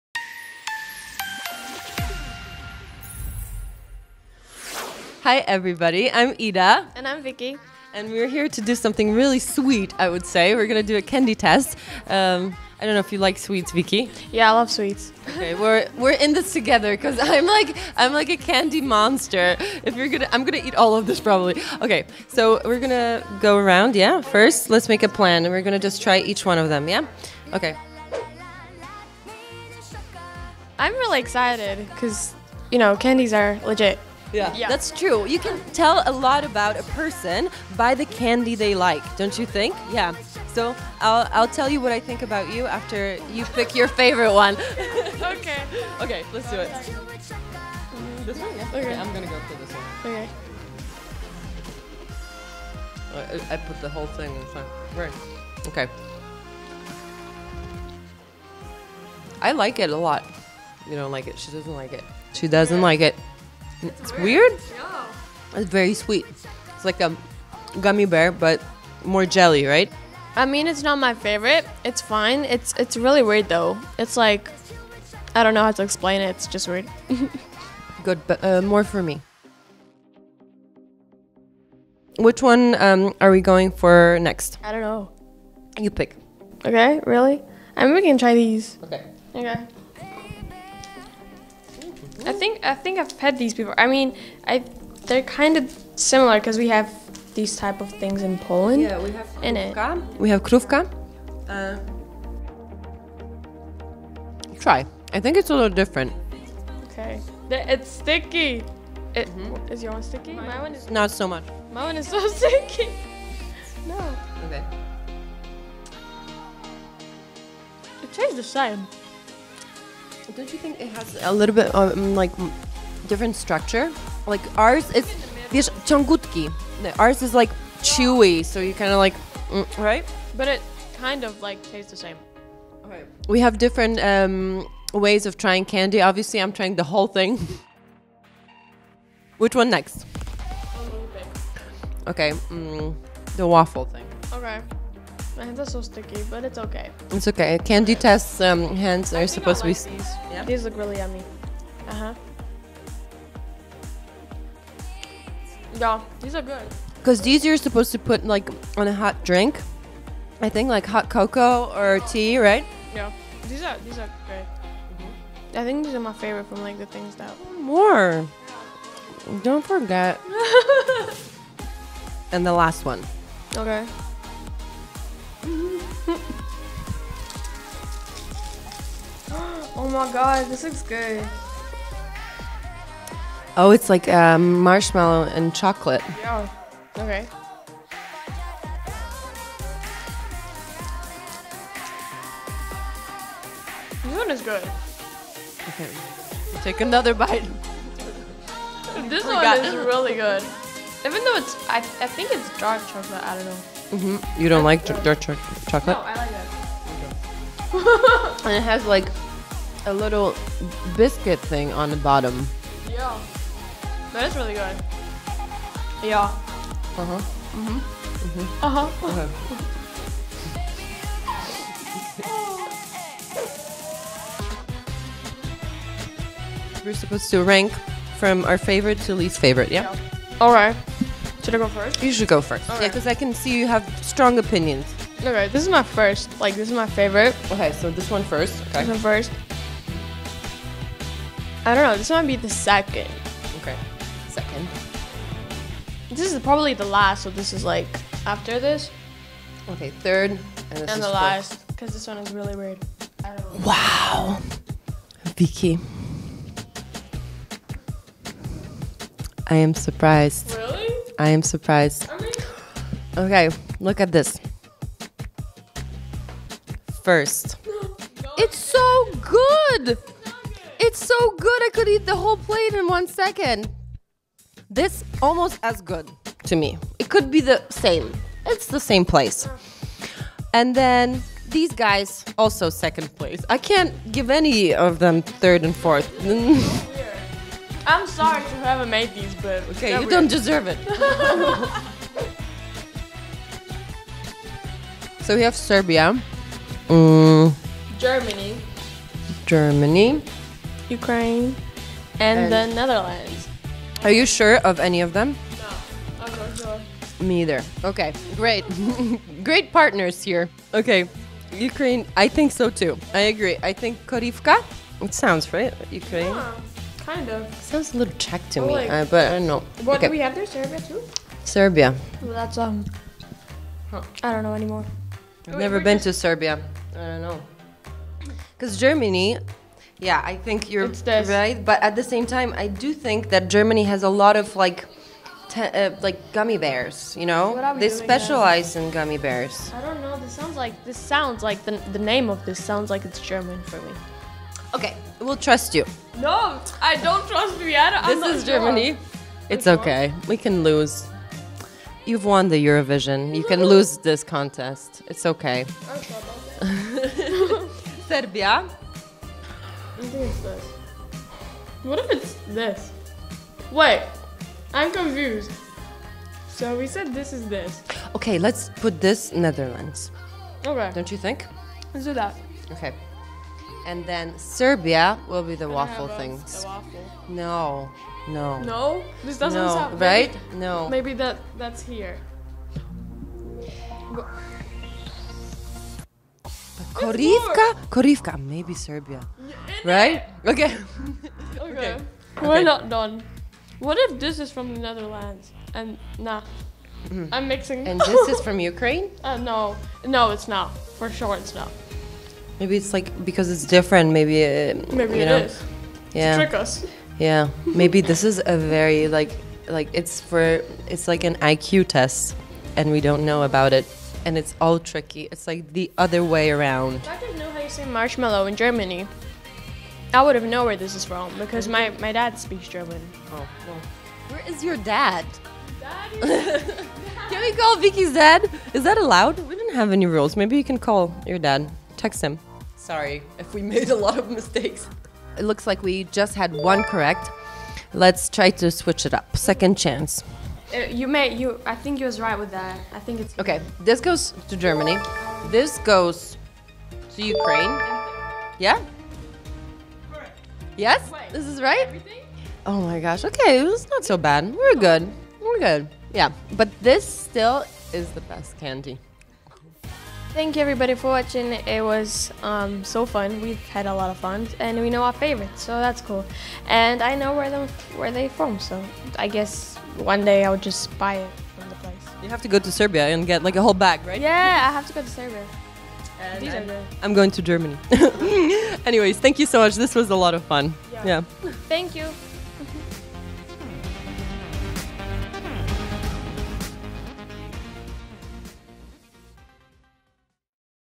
Candy Test with Viki Gabor & Ida Nowakowska-Herndon - Part 2 - Junior Eurovision 2020
In the second part of the Junior Eurovision Candy test we bring four new candies to the table. This time it's the Junior Eurovision 2019 winner Viki Gabor and Junior Eurovision host Ida Nowakowska-Herndon, who takes on the challenge.

They both love sweets but during the game it turns out that their taste in candy separates them. In the end they have to guess which candy belongs to which country. Will they be able to beat the result of Ala and Ola Tracz from the first episode of the Junior Eurovision Candy Test?